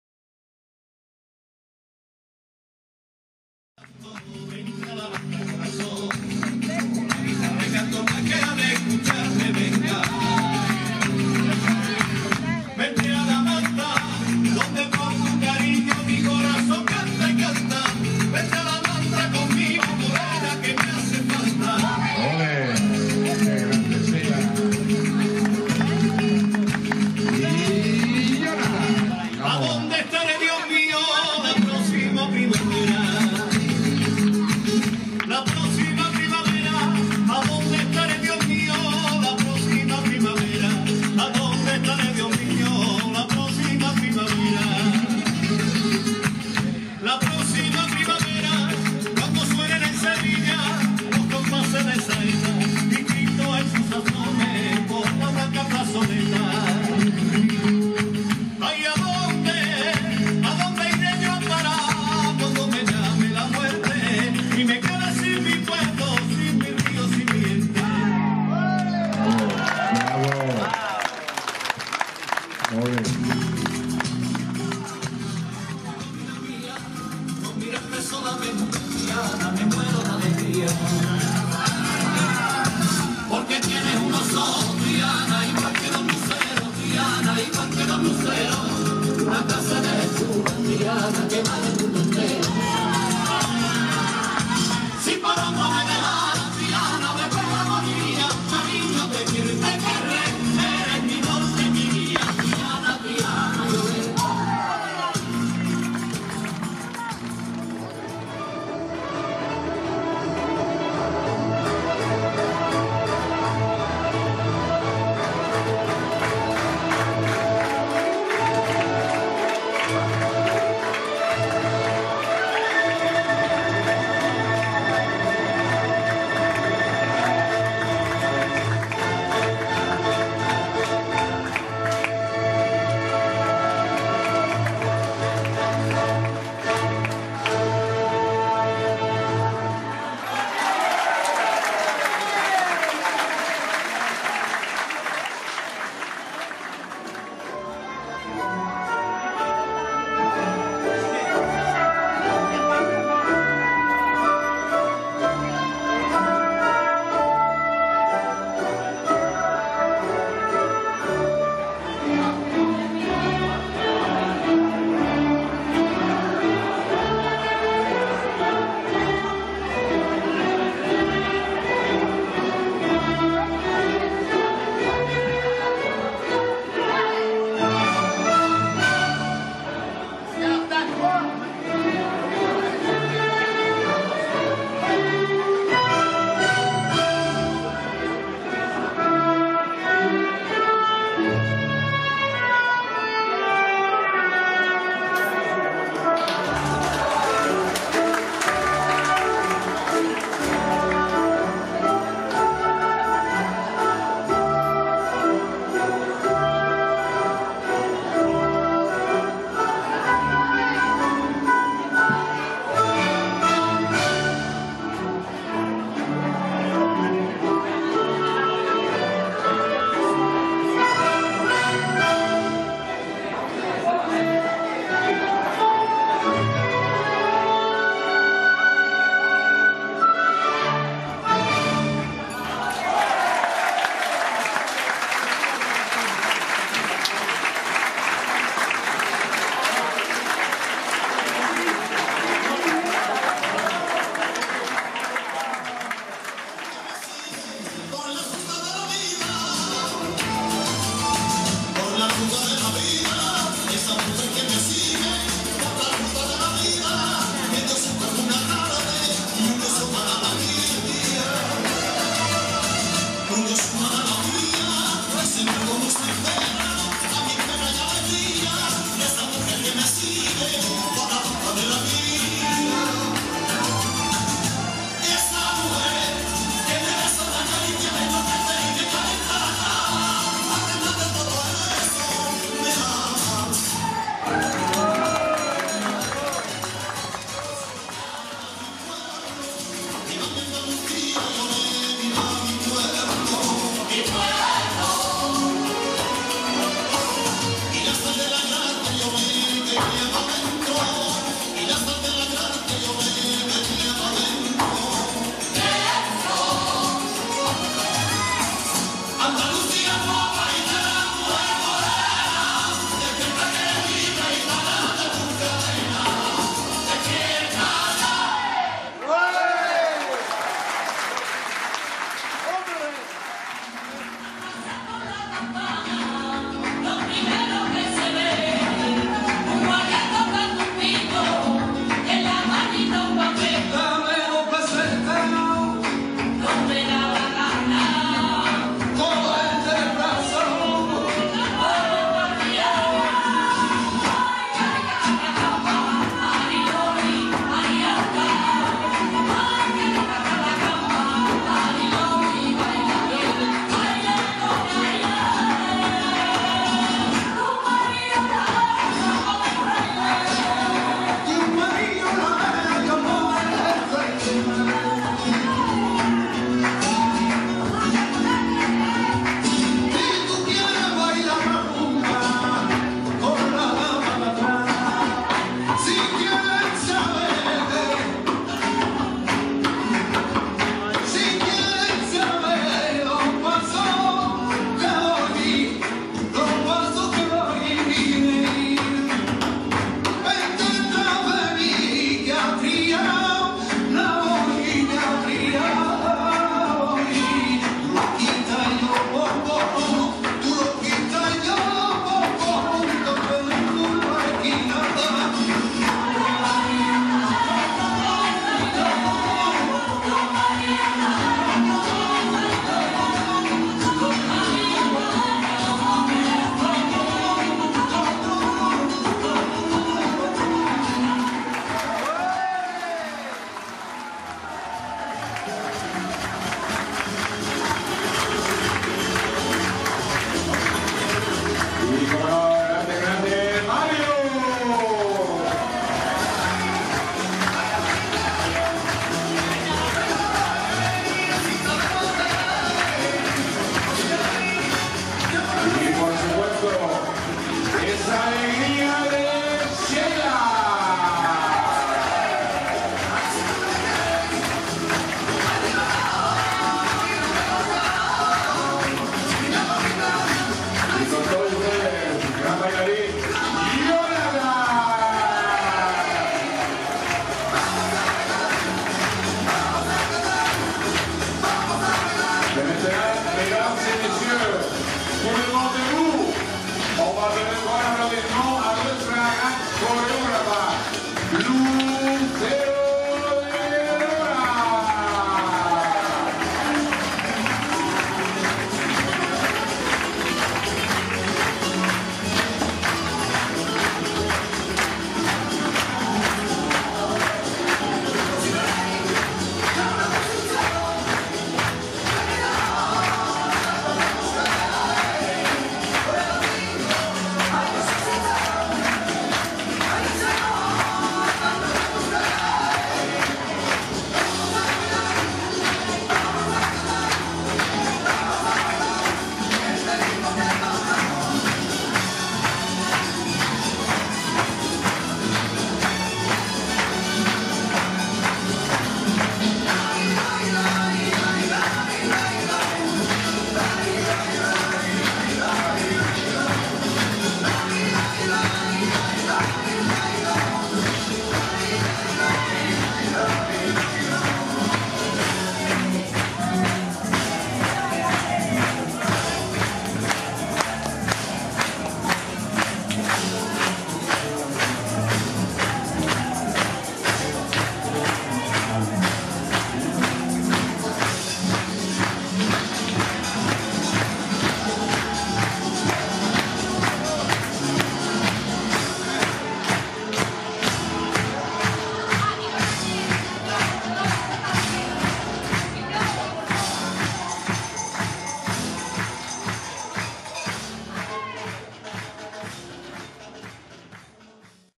Avec Damien POYARD en Espagne. Magie Andalouse
Vidéo enregistrée à la Masia Tordéra. La danse des chevaux Andalous et groupe de danseurs flamenco.